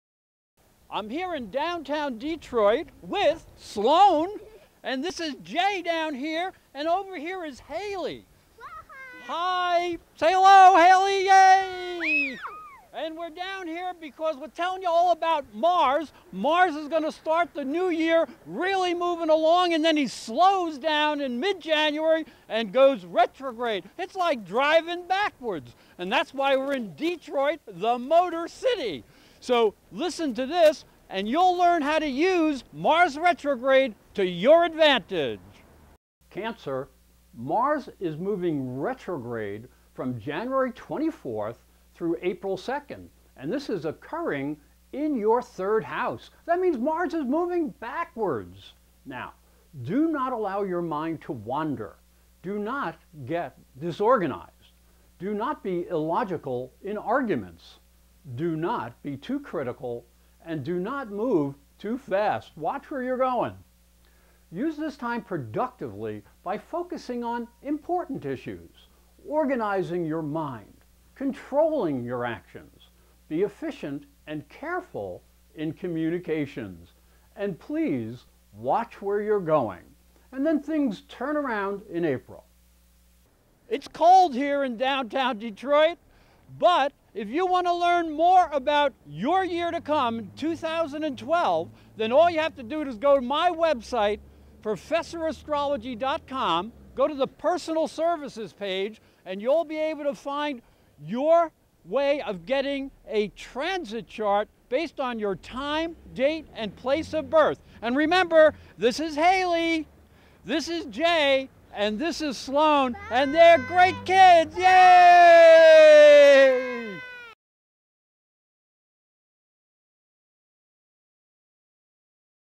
Cancer Astrology January 2012
presents Cancer Astrology 2012 Horoscope - Cancer, Mars transits Virgo and goes retrograde in your third house from January 24, 2012 till April 14, 2012.   This is an important time for you to reorganize your mind and thinking processes.  Be prepared to make a change in the way you analyze data and communicate to others.  Take care in travel.  Here is the astrological horoscope and prediction for Cancer for January 2012.  You can register for free astrology predictions at ProfessorAstrology.com This movie was filmed in Detroit, Michigan.